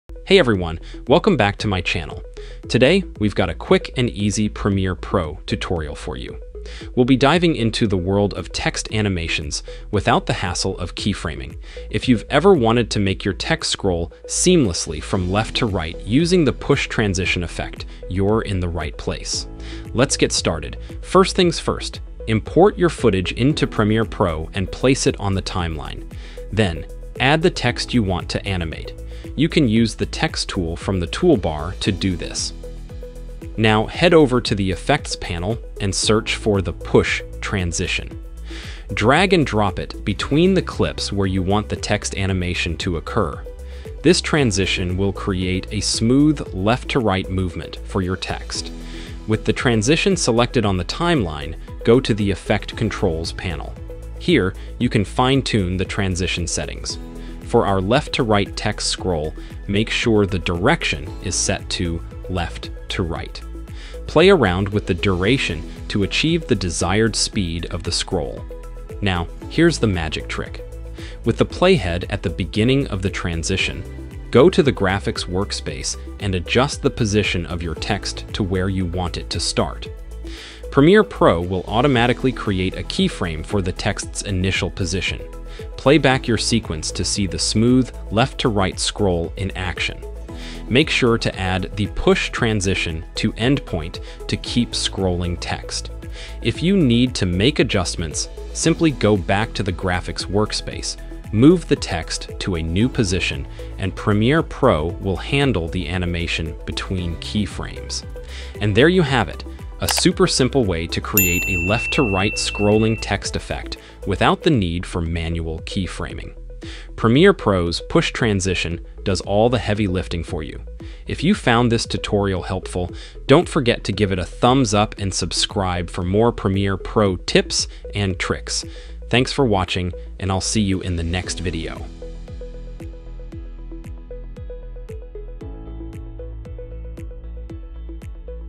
How to Scroll Text Left to Right without Keyframing in Premiere Pro | Premiere Pro Tutorials
Learn the art of seamless text scrolling in Premiere Pro 2023 without the hassle of keyframing. This tutorial will guide you through an efficient method to achieve left-to-right text movement, enhancing your video editing skills. Save time and frustration with this technique that's perfect for dynamic titles and engaging content. Watch now to elevate your Premiere Pro game!
How to scroll text left to right without keyframing in Premiere Pro 2023